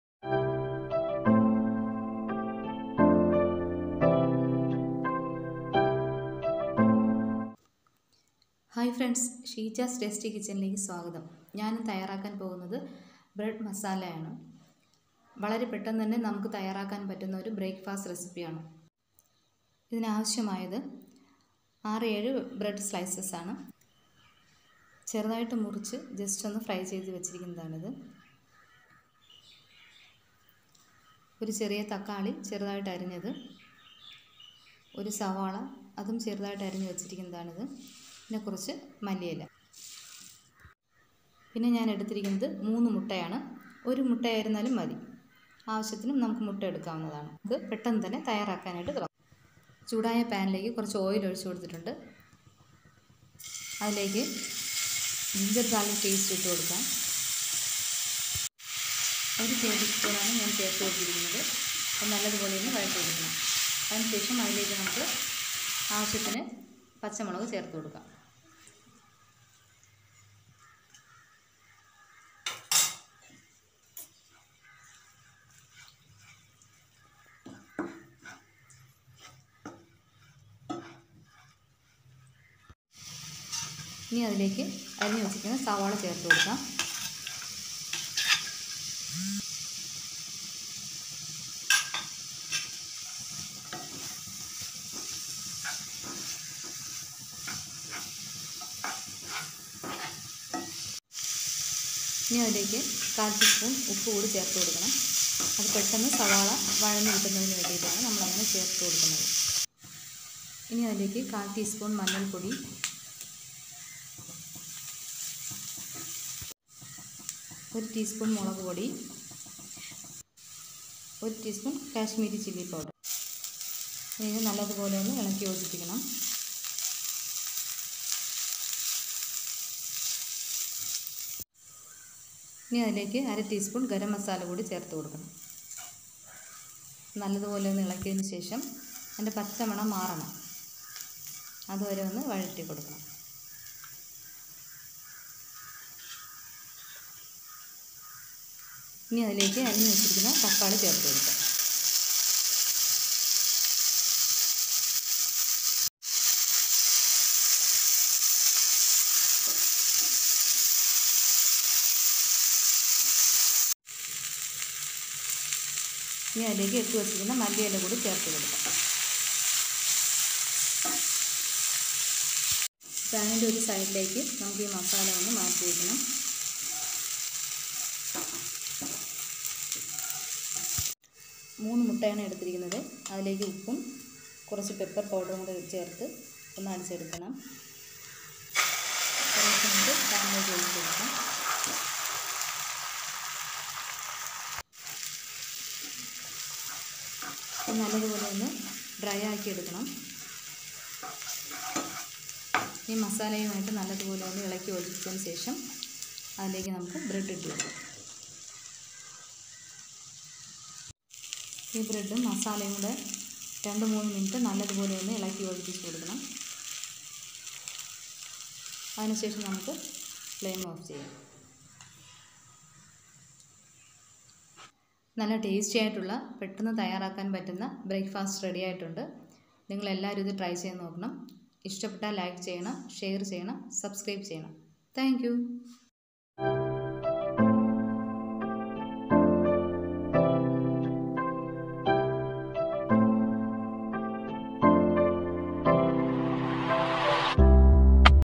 Bread Masala | Easy & Healthy Breakfast Recipe | Bread Recipes || Sheeja's Tasty Kitchen
Tomato - 1
Onion - 1
Coriander leaves -
Egg - 3
Oil - 2tbsp
Ginger - garlic paste - 1tsp
Green chilli - 1/2tsp
Salt -
Pepper powder - 3pinch